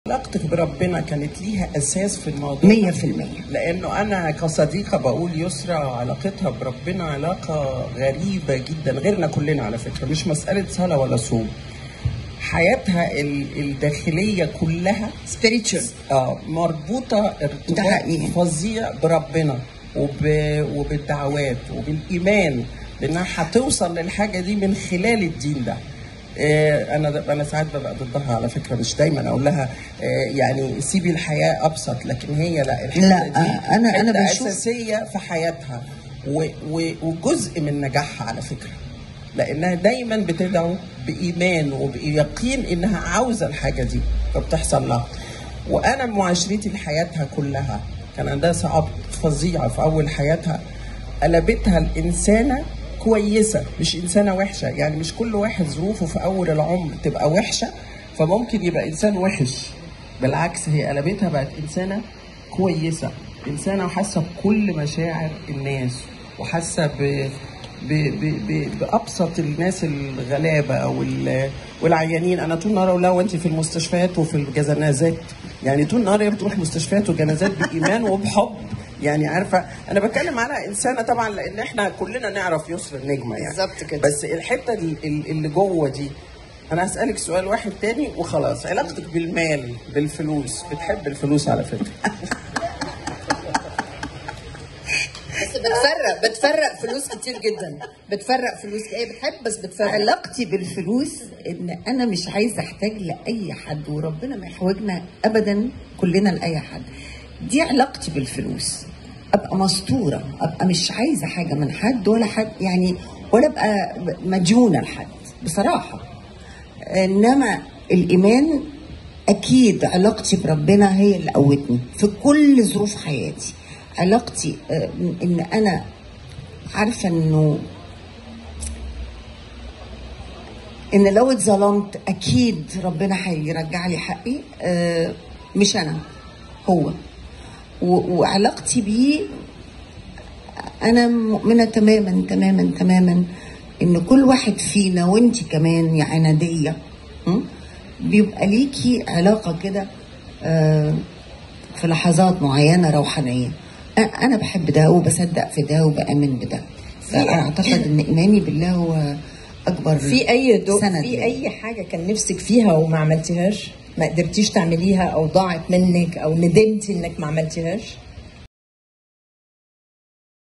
الجونة السينمائي.. إيناس الدغيدي تسأل يسرا عن علاقتها بالدين والمال.. والأخيرة تجيب
تقرير: منى الموجي
تصوير : إسلام فاروق
مونتاج : محمد محسن